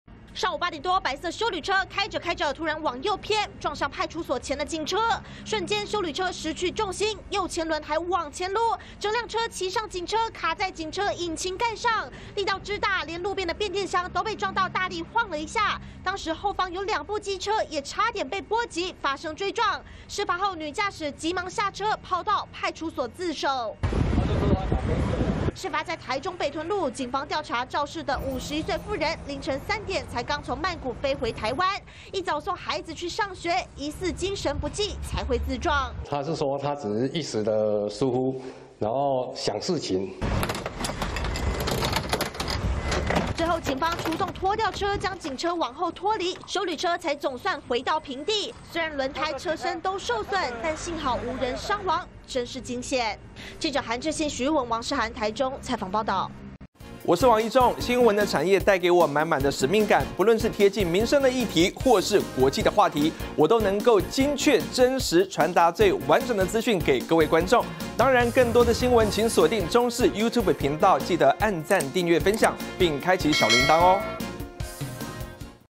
驚魂!凌晨返台又載小孩上學 女駕駛恍神"騎上警車"|中視新聞 20230103
邊境解封， 一名51歲婦人，凌晨三點才剛從泰國曼谷，搭紅眼班機飛抵台灣，隨後她又開車送孩子去上學，疑似精神不濟，整輛車開去撞派出所門口的警車，還騎上去，路邊2名騎士也差點被波及，險釀成追撞，超級驚險。